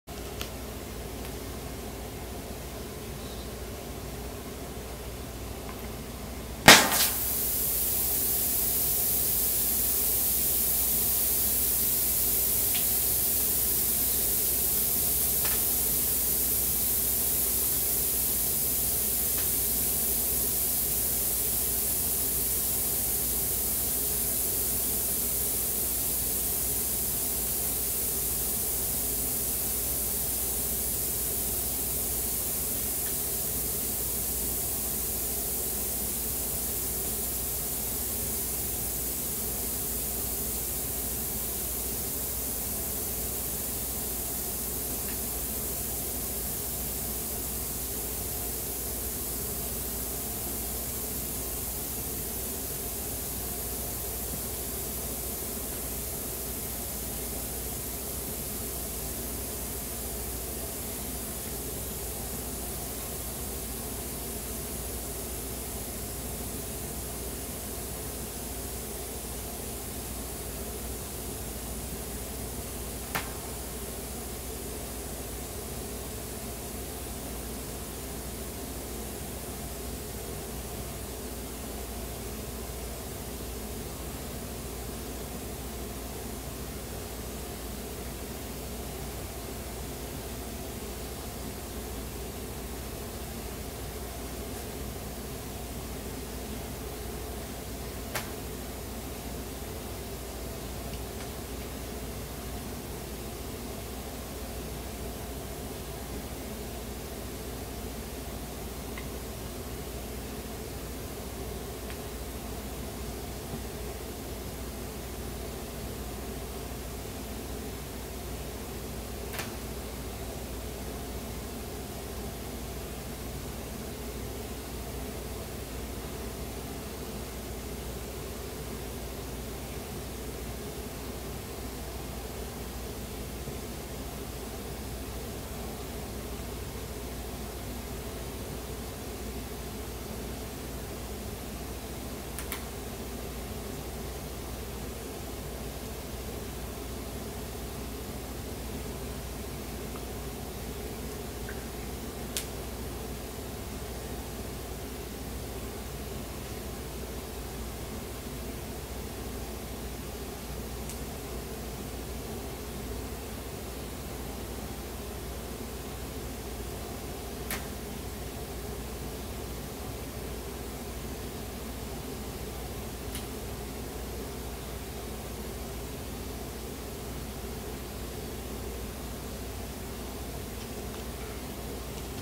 Popping Blue Inflatable Shark
Popping a hole in a blue inflatable shark and letting it deflate ASMR.